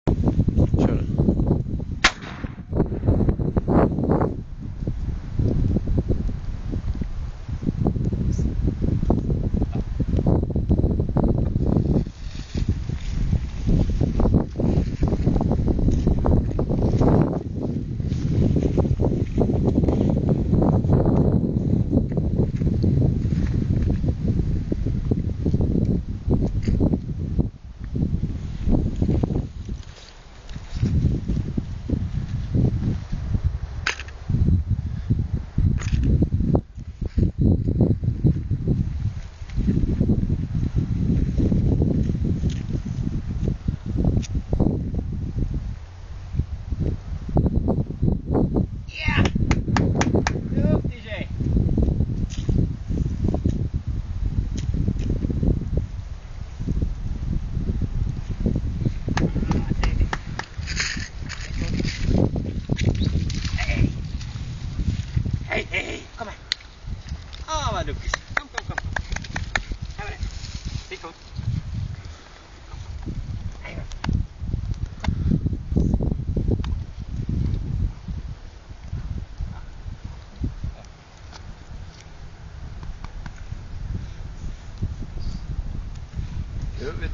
Jocke och Yazza apporterar
Münsterländern Yazza (10 månader gammal) apporterar .